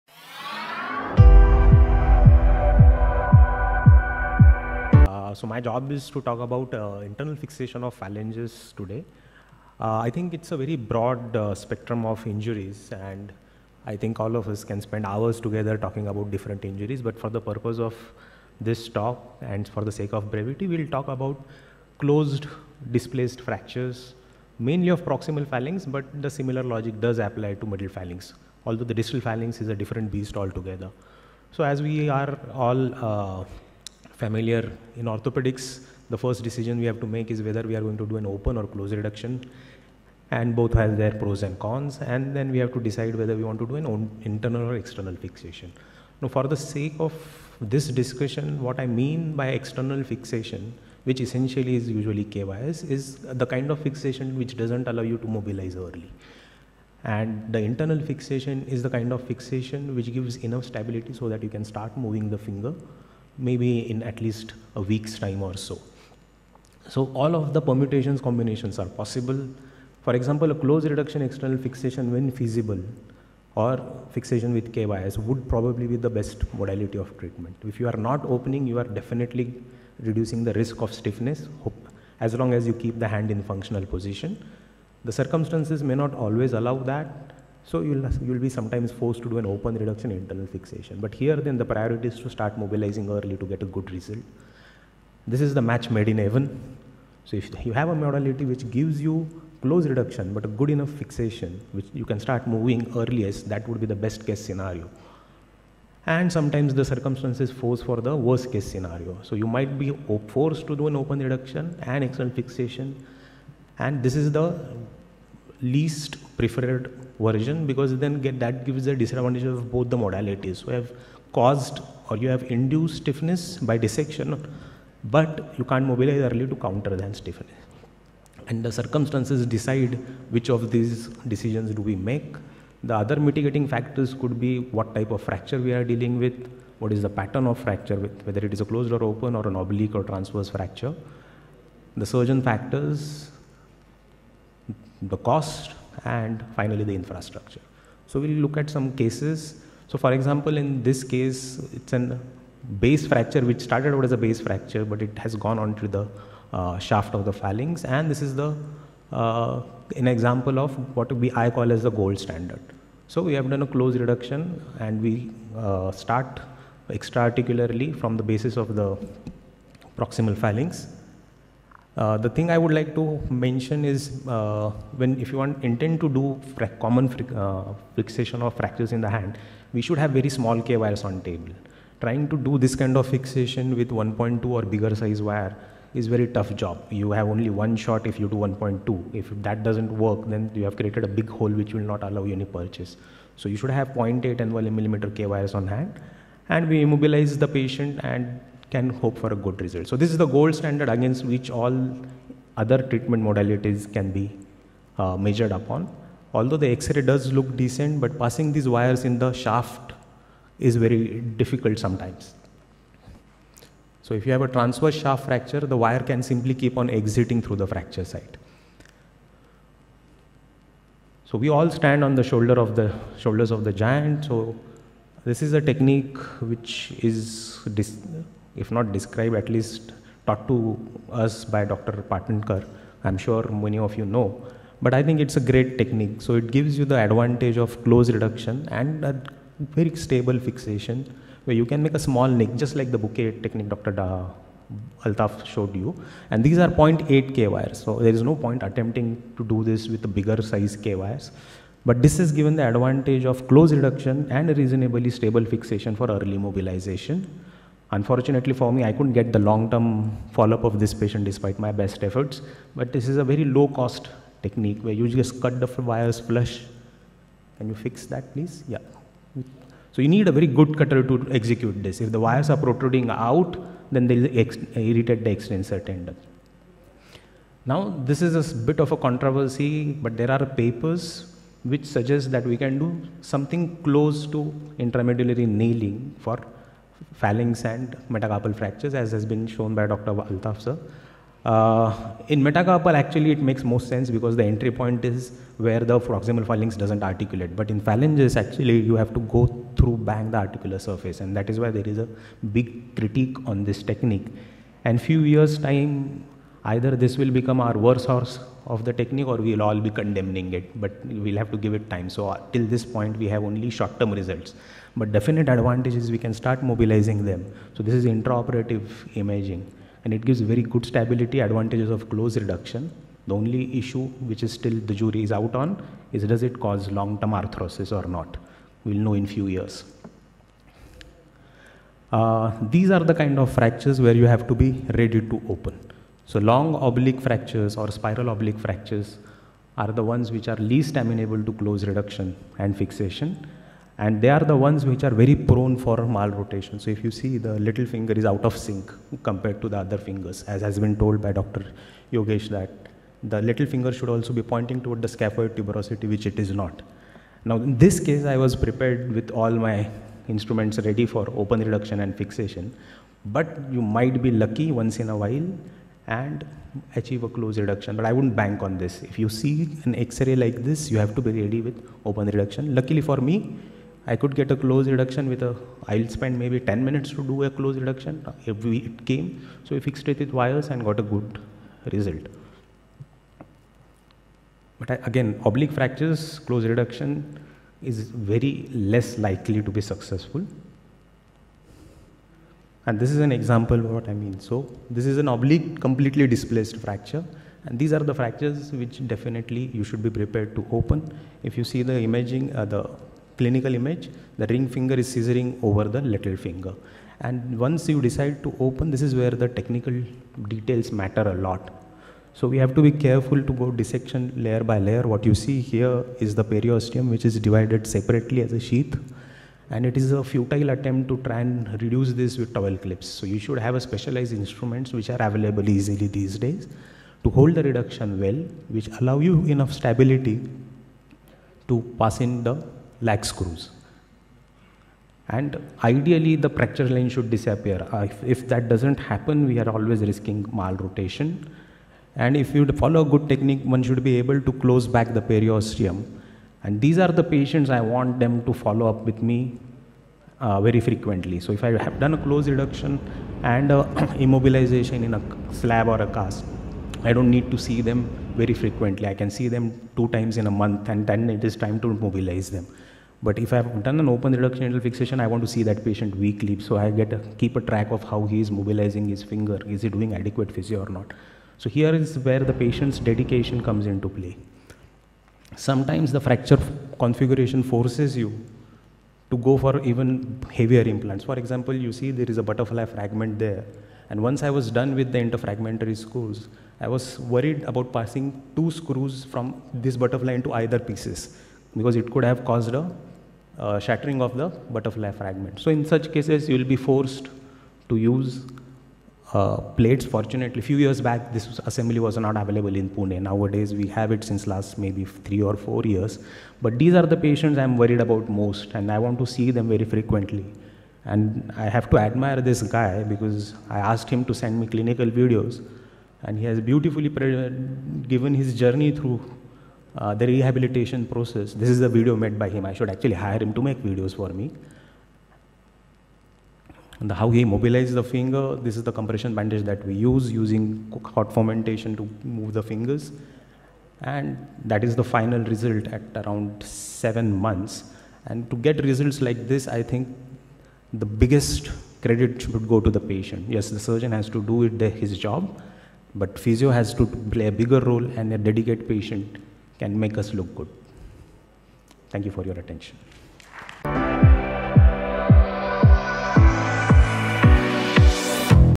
POSAC 2024 : Internal Fixation of Phalanges - Dr Nilesh Darawade
This Talk is Part of Pune Orthopaedic Society Annual Conference: POSAC 2024

President POS & Organizing Chairman POSAC 2024
Dr. Arjun Viegas

Secretary POS & Organizing Secretary POSAC 2024
Dr. Yogesh Khandalkar